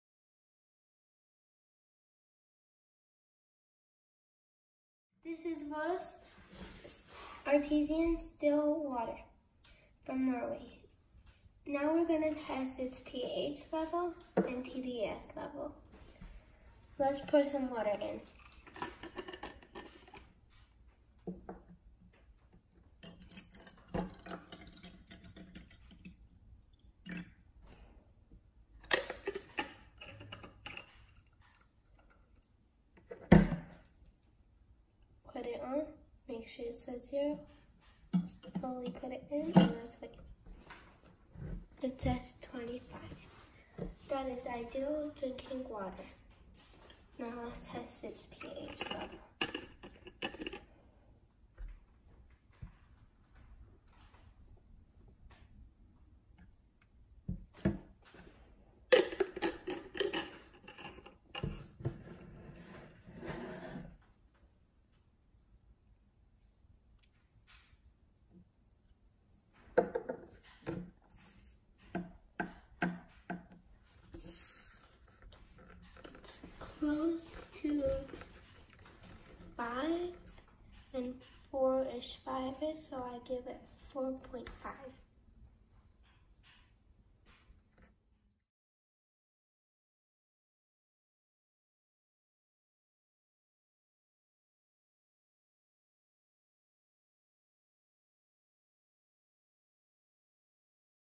VOSS, From Norway Water Test - pH and TDS
VOSS Water Test - pH and TDS
VOSS Water from Norway.
If you'd like to buy this water from Amazon, please

Results:
pH: 4.5
TDS: 25

AquaION pH test kit
Xiaomi TDS Tester